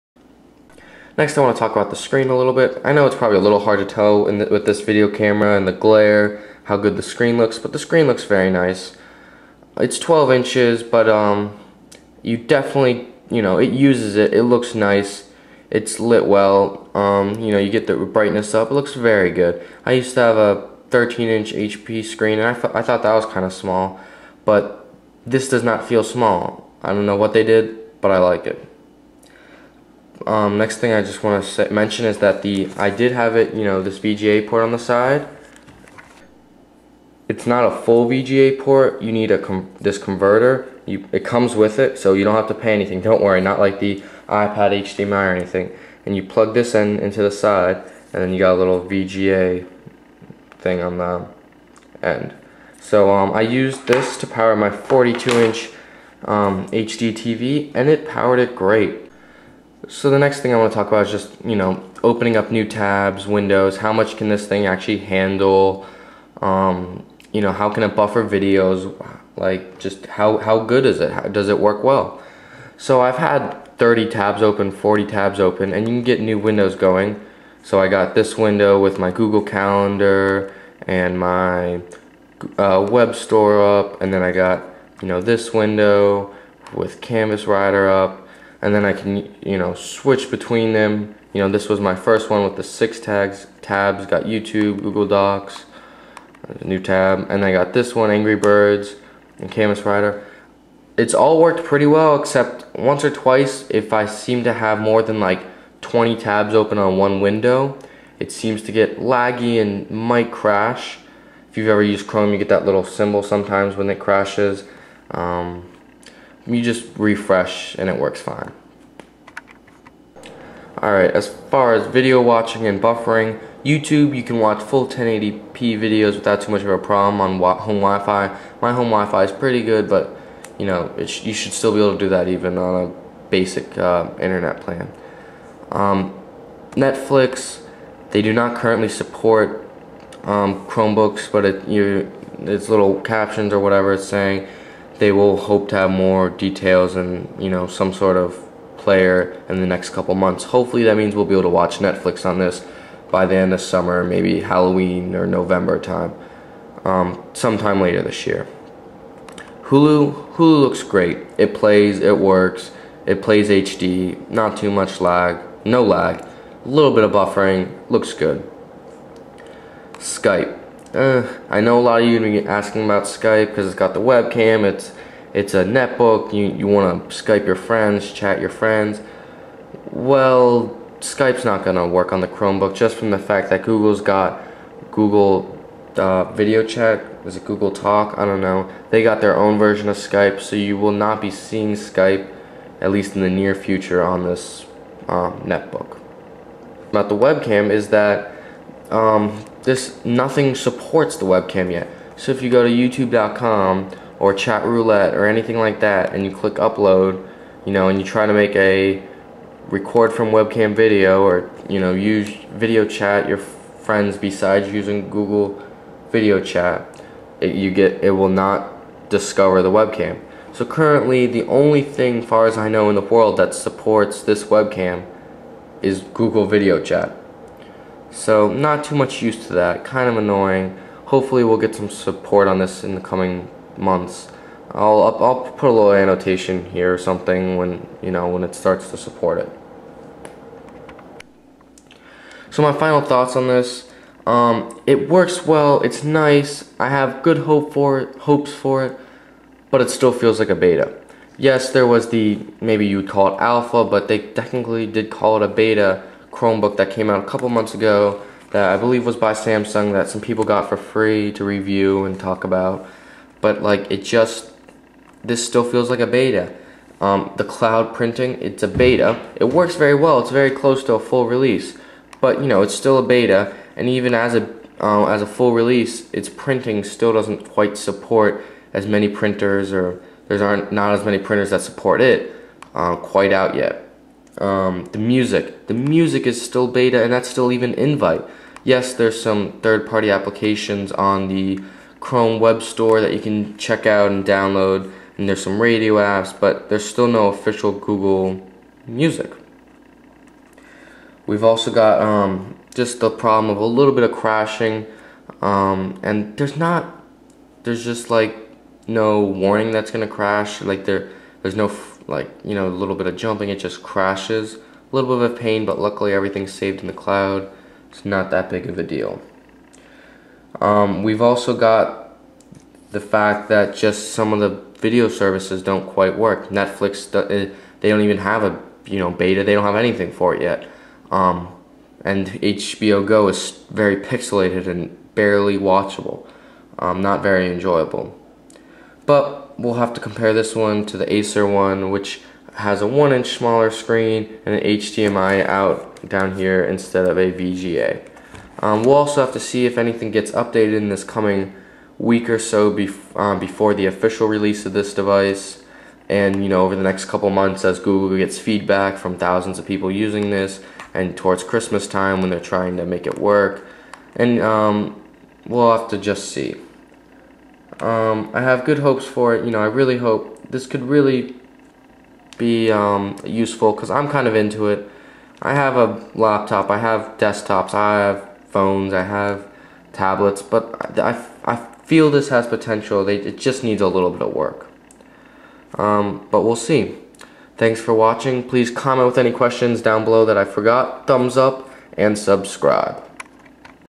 Samsung series 5 chromebook review and hands on (part 2)
Set to to come out June 15ht

specs
Processor: 1.66 GHz Intel Atom dual-core N570 processor
Storage: 16 GB solid-state drive (SSD)
System Memory: 2 GB
Communications: Wireless-N Wi-Fi (802.11b/g/n), integrated 3G (via optional Verizon Wirelesssubscription)
Ports: Two USB 2.0, 4-in-1 memory card reader (SD, SDHC, SDXC, MMC)
Video out: VGA (via optional dongle)
Audio: Built-in stereo speakers, combo headphone/microphone jack
Battery: 6-cell battery for up to 8.5 hours of use
Webcam: 1-megapixel

No bluetooth